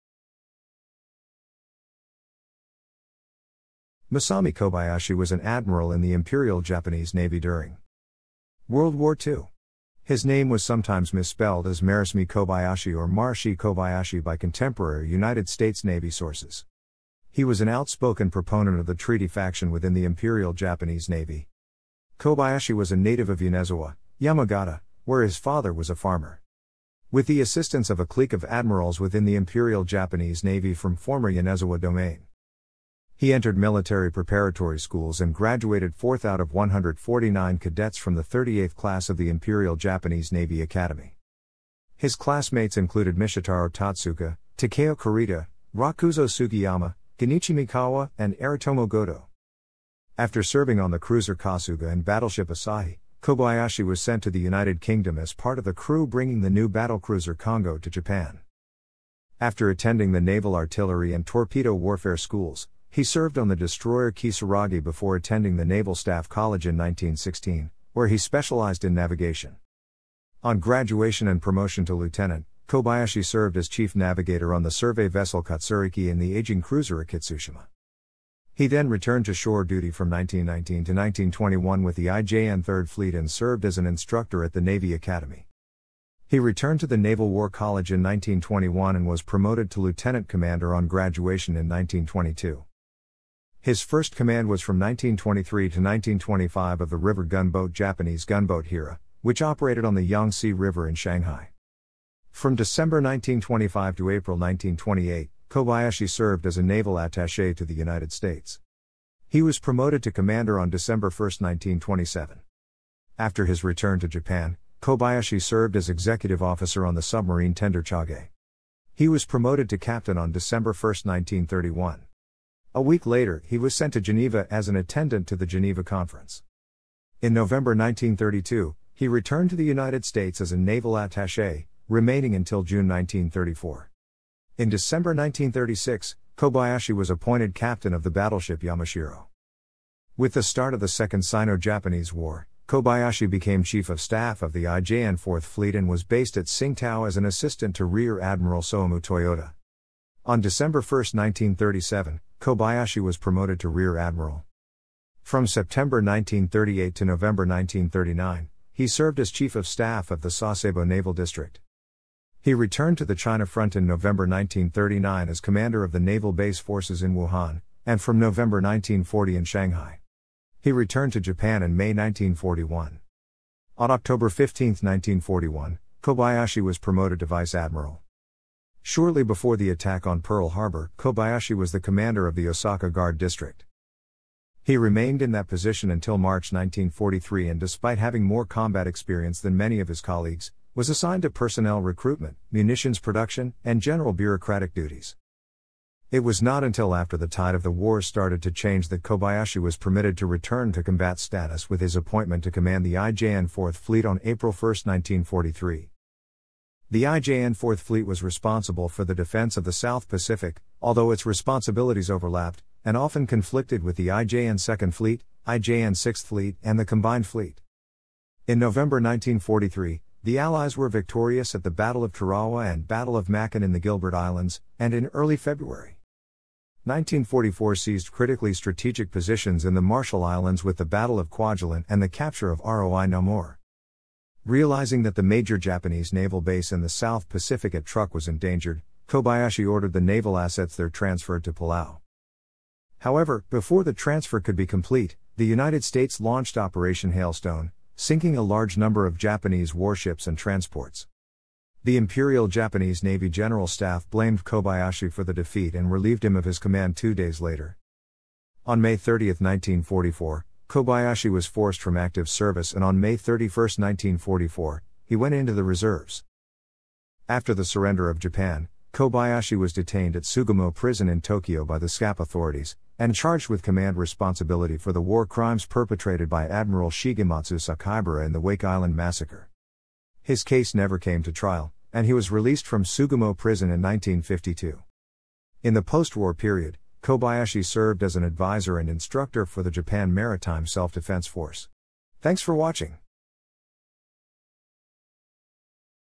Masami Kobayashi
Masami Kobayashi （小林仁, Kobayashi Masami or Kobayashi Masashi, 18 June 1890 – 7 August 1977） was an admiral in the Imperial Japanese Navy during
World War II. His name was sometimes misspelled as "Marasmi Kobayashi" or "Marashi Kobayashi" by contemporary United States Navy sources.
He was an outspoken proponent of the Treaty Faction within the Imperial Japanese Navy.
Kobayashi was a native of Yonezawa, Yamagata, where his father was a farmer.
With the assistance of a clique of admirals within the Imperial Japanese Navy from former Yonezawa Domain （including Yamashita Gentarō and Chūichi Nagumo）,
he entered military preparatory schools and graduated 4th out of 149 cadets from the 38th class of the Imperial Japanese Navy Academy.
His classmates included Michitaro Totsuka, Takeo Kurita, Rokuzō Sugiyama, Gunichi Mikawa and Aritomo Gotō.
After serving on the cruiser Kasuga and battleship Asahi, Kobayashi was sent to the United Kingdom as part of the crew bringing the new battlecruiser Kongō to Japan.
After attending the naval artillery and torpedo warfare schools, he served on the destroyer Kisaragi before attending the Naval Staff College in 1916, where he specialized in navigation.
On graduation and promotion to lieutenant, Kobayashi served as chief navigator on the survey vessel Katsuriki and the aging cruiser Akitsushima.
He then returned to shore duty from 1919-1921 with the IJN 3rd Fleet and served as an instructor at the Navy Academy.
He returned to the Naval War College in 1921 and was promoted to lieutenant commander on graduation in 1922.
His first command was from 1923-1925 of the river gunboat Japanese gunboat Hira, which operated on the Yangtze River in Shanghai.
From December 1925 to April 1928, Kobayashi served as a n...